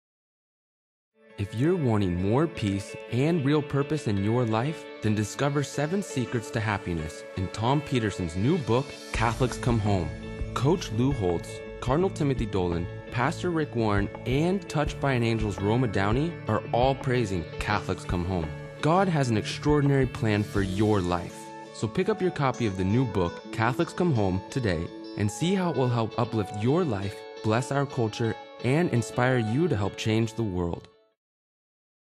Catholics Come Home Book Ad with Endorsements
This short commercial promotes Tom Peterson's new book, Catholics Come Home: God's Extraordinary Plan for Your Life, encouraging people to learn the seven secrets to happiness, peace, and purpose. It includes endorsements from Coach Lou Holtz, Cardinal Timothy Dolan, Pastor Rick Warren, and Actress Roma Downey.